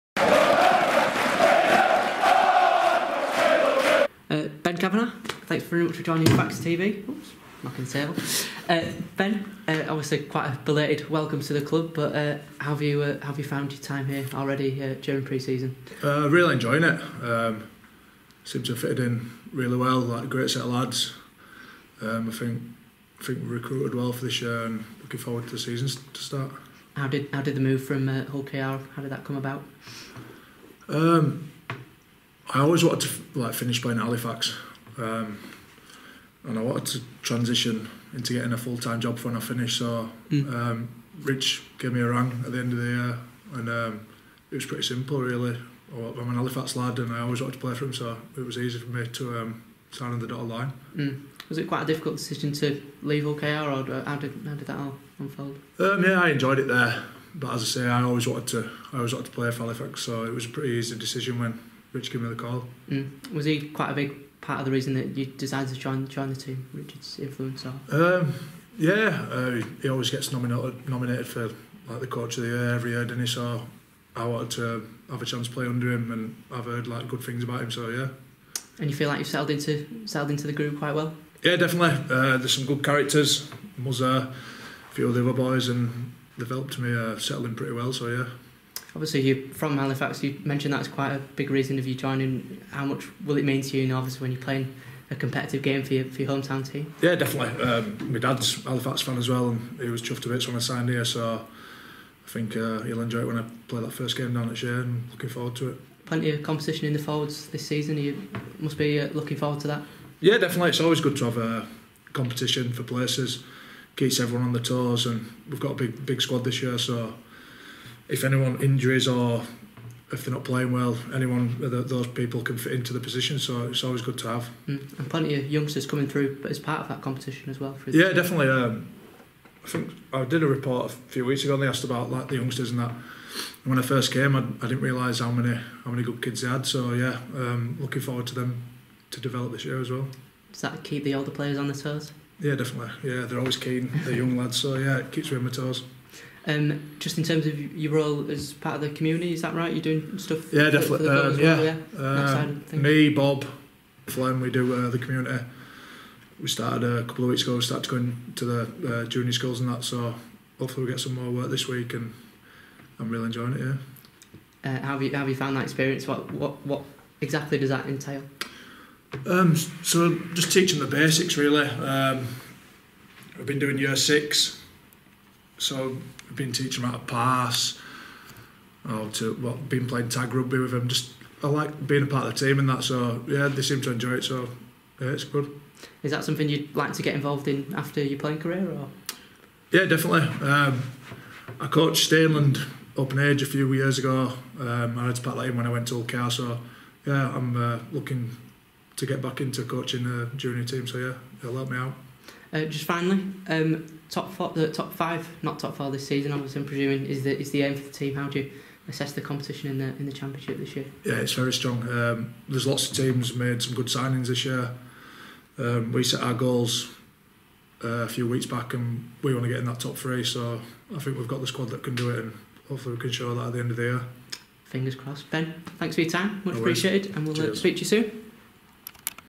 Introducing Ben Kavanagh | Scotland international forward Kavanagh joins from Hull KR
FaxTV chatted to Scotland international forward Ben Kavanagh after he joined Halifax from Hull KR.